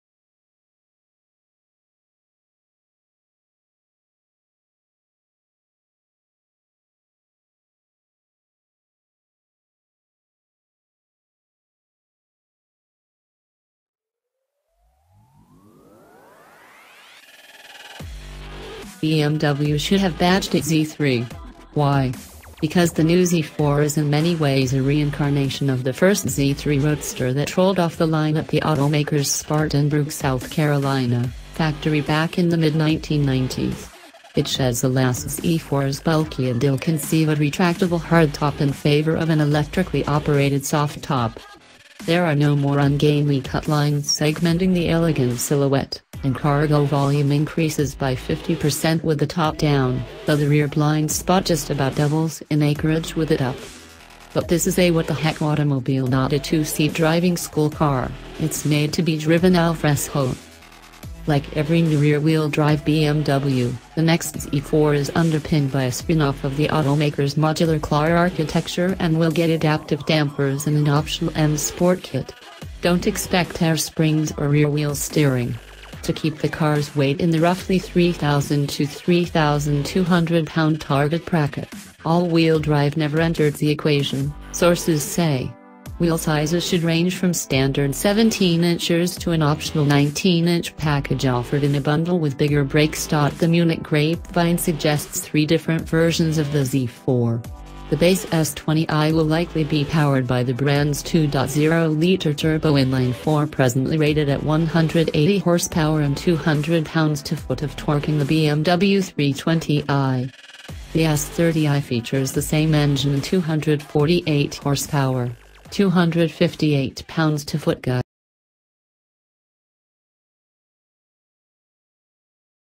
WOW AMAZING !!! 2019 BMW Z4 Reveal Spec & Price
WOW AMAZING !!! 2019 BMW Z4 Reveal Spec & Price - The third-generation BMW Z4 will be introduced later this year, and we’ve had our best glimpse yet at what the production variant of the Audi TT rival might look like in revealing new spy images.

The BMW Z4 made its global debut at the 2017 Pebble Beach Concours d’Elegance, but it wasn’t until later that year, at the Frankfurt Motor Show, that the concept for the BMW roadster was revealed to a European audience. The concept car shown at the two events revealed what the 2018 Z4 will look like.

██████ GET DAILY UPDATES! NOW! ██████